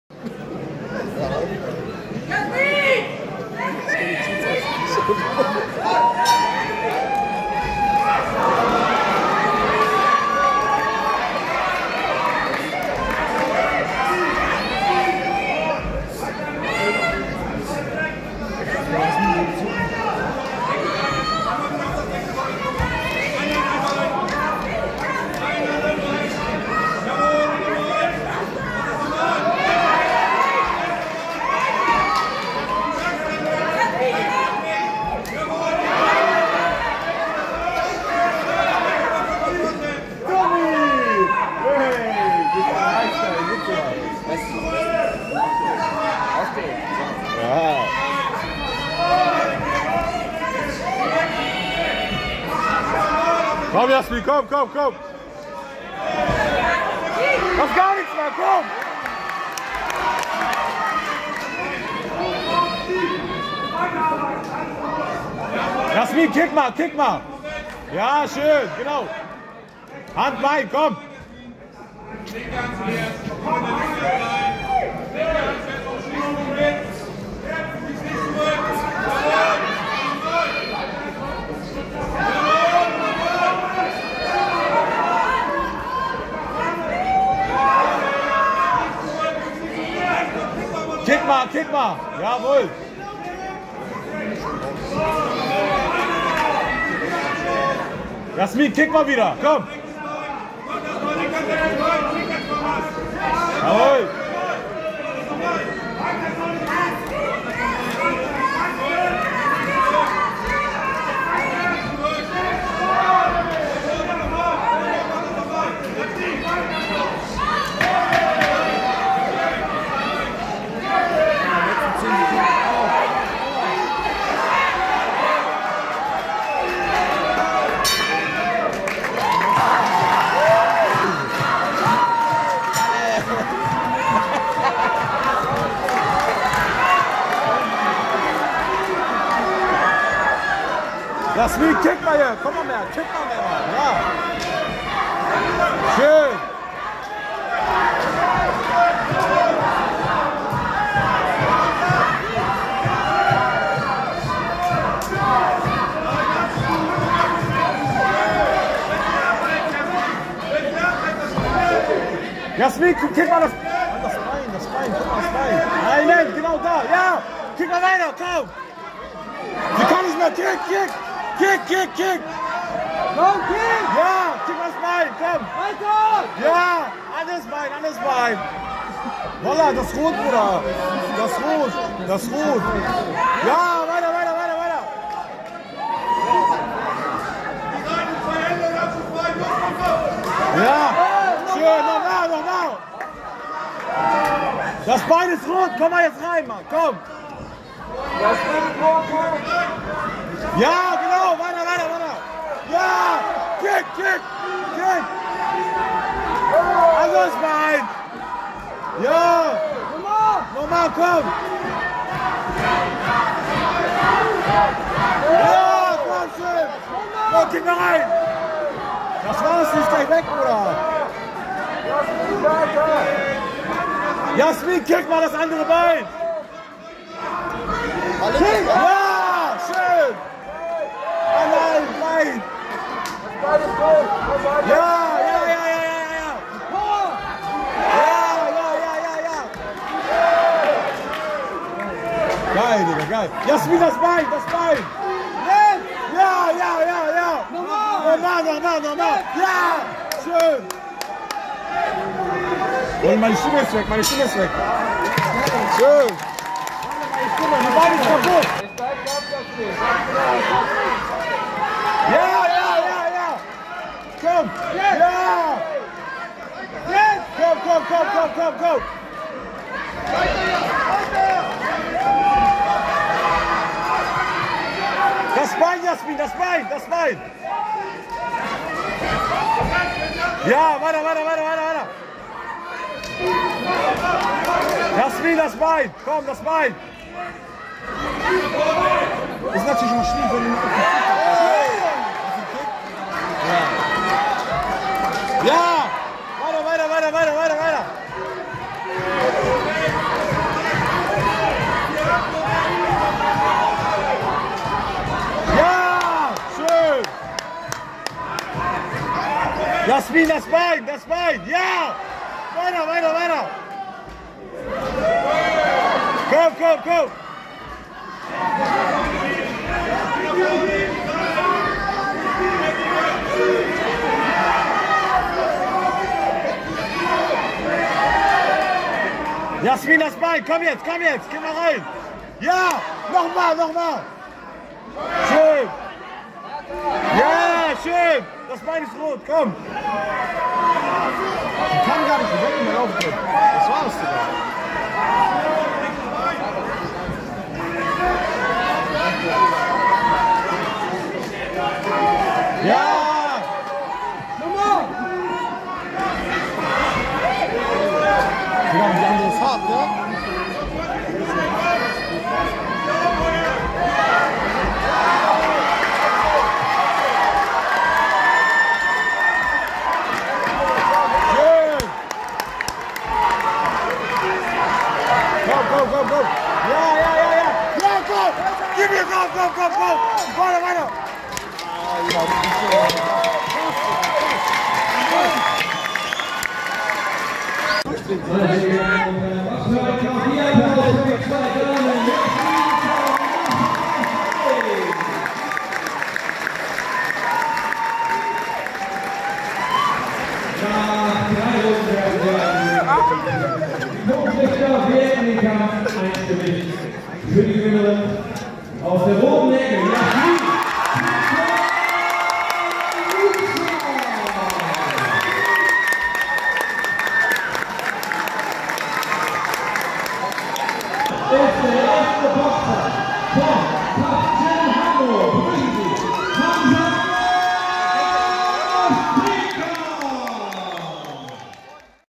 KICKMAS THAI BOX GALA 2022 - JASMIN UCAR VS MASCHA SEITZ
Hamburg. Jasmin Ucar tritt bei der Kickmas Thai Box Gala 2022 in der Turnhalle Wandsbek gegen Mascha Seitz an.